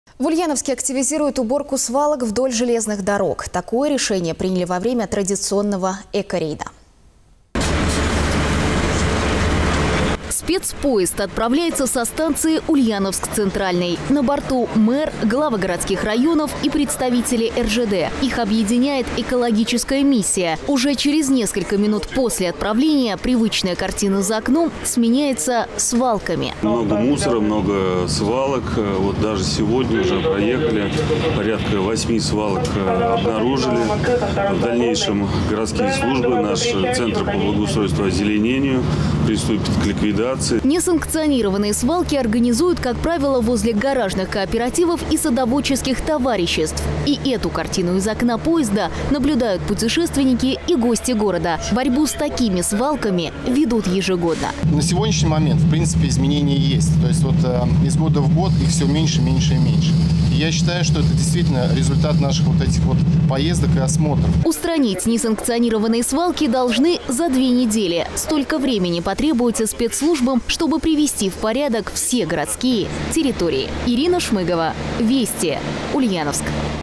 В Ульяновске активизируют уборку свалок вдоль железных дорог
Такое решение приняли во время традиционного экорейда.